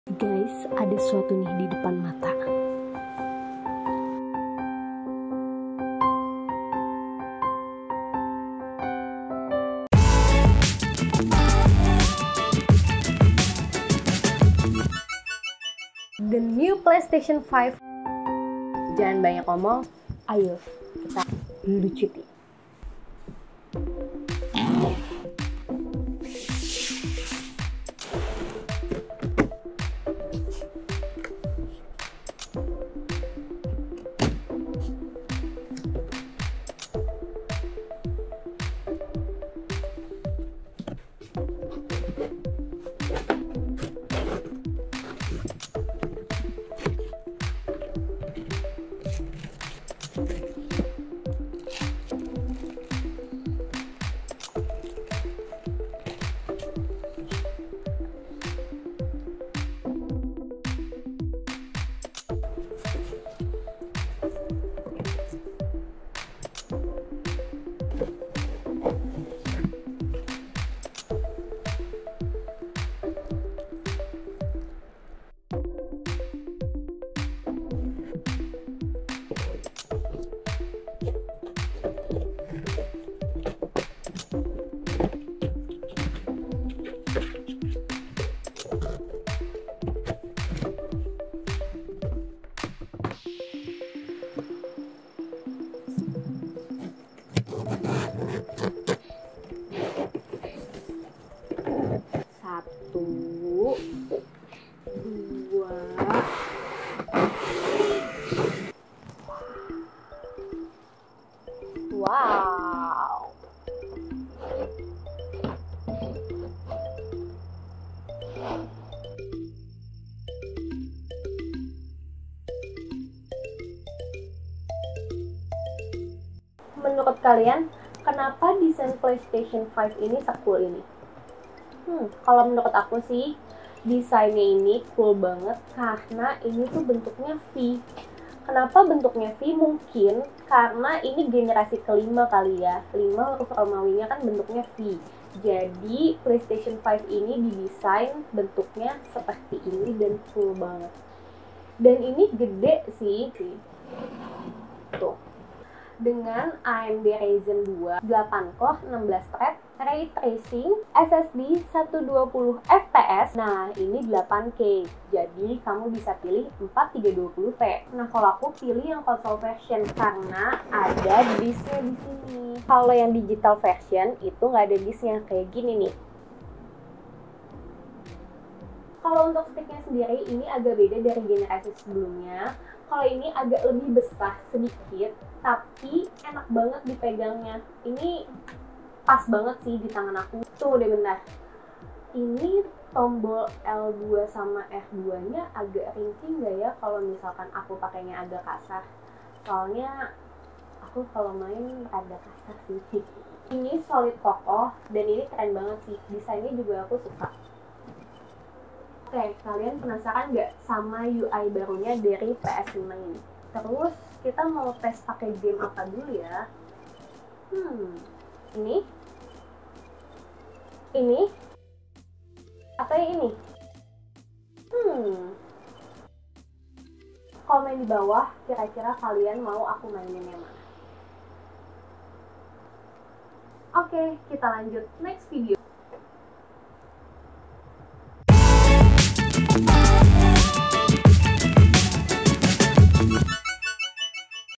PEREMPUAN INDONESIA PERTAMA YANG PUNYA PLAYSTATION 5 - PS5 FINALLY
Hai semuanya. Kali ini aku bakal unboxing PS5!
Wah bakal jadi perempuan indonesia pertama kayaknya yg bakal unboxing PS5!

Kira2 aku harus mainin game apa ya buat test PS 5 baru aku iniii?

Comment di bawah yaaaa